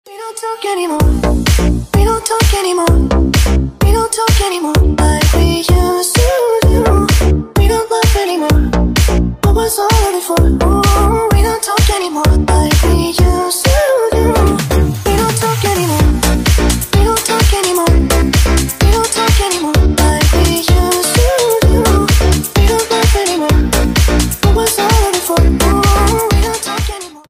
Apartamento com 114m² e 2 Suítes ✨ | Porto Alegre - RS
Mais informações sobre o imóvel:
📍 Bairro Rio Branco;
📐 114m² de área privativa;
🛌 02 suítes;
🚗 02 vaga.

🖥️ Código: 27967